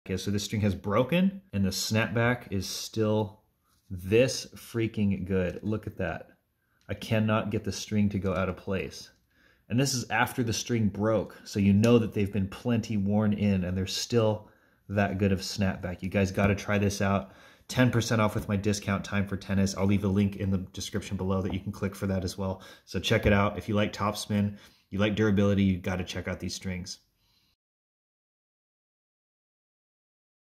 Don't Miss Out on This String
I can't say enough good things about ReString - Zero

Eco friendly and awesome performance! I've tried just about everything but try for yourself and see.